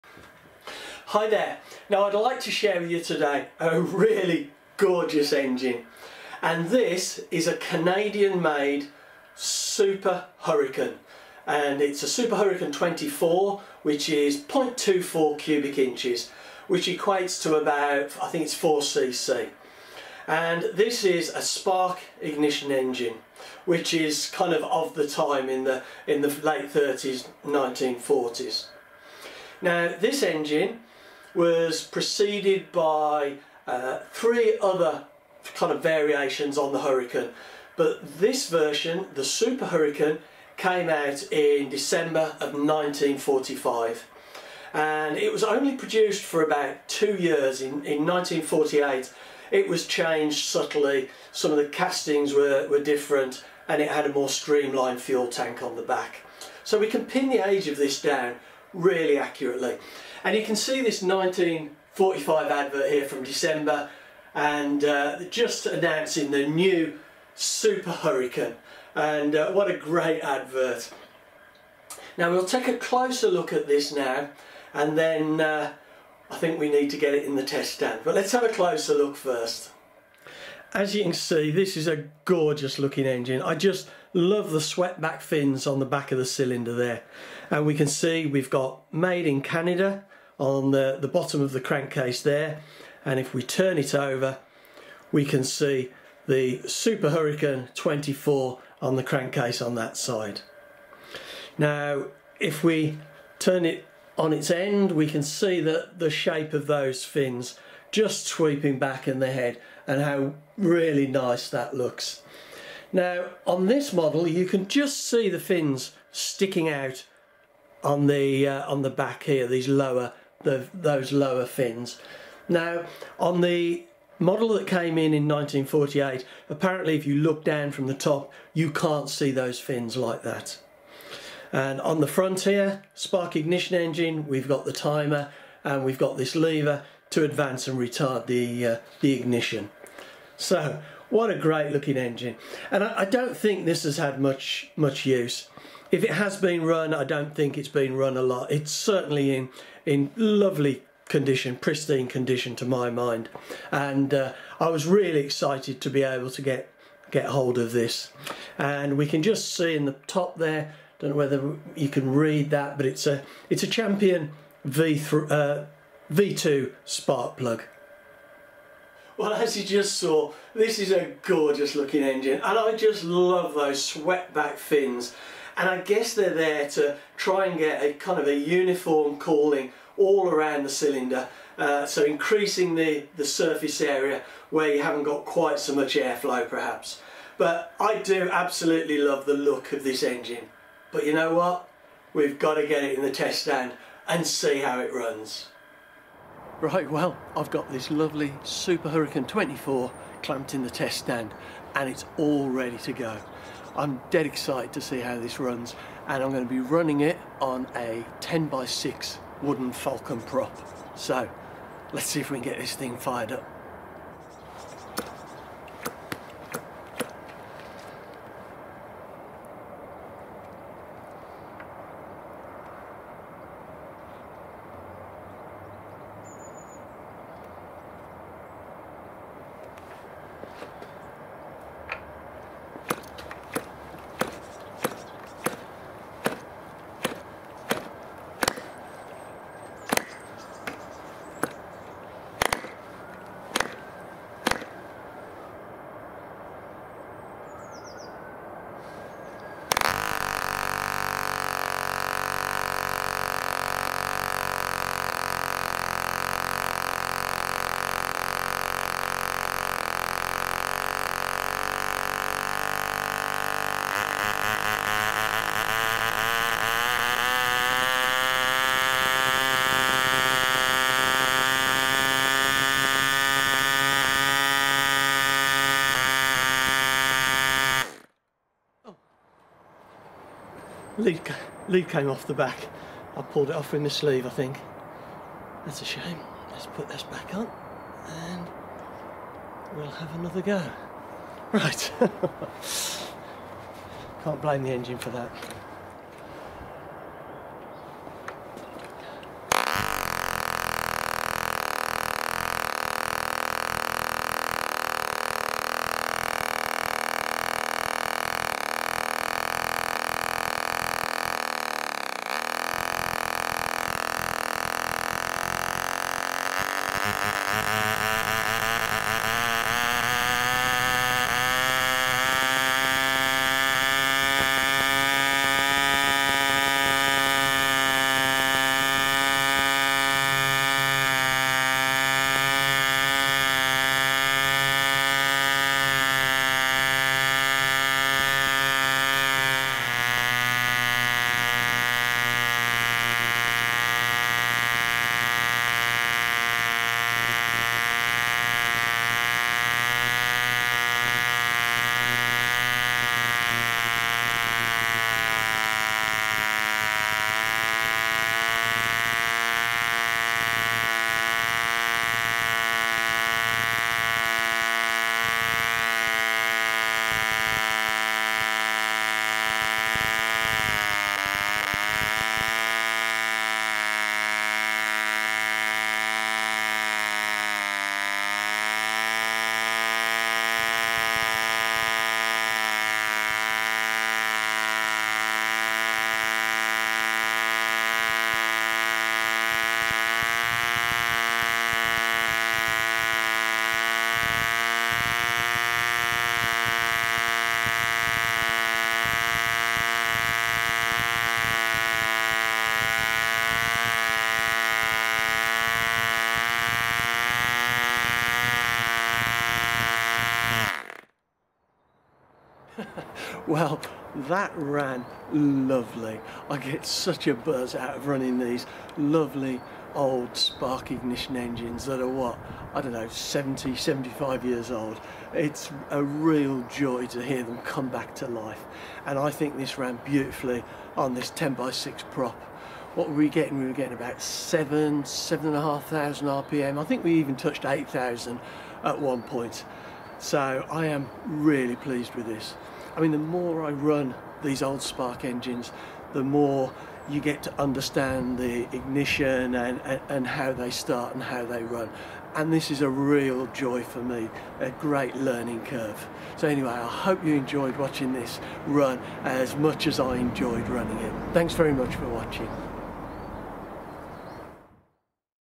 Super Hurricane 24 Spark Ignition Engine - Vintage 1940's Petrol Engine
A Canadian Super Hurricane 24, designed by Ray Hunter in Toronto,  This version of the engine was only produced for little over a two year period between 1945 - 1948.  The engine is running with a Champion V2 spark plug and Coleman fuel, a petroleum fuel, similar to White Gas and SAE 50 oil, in a mix of 3:1, respectively.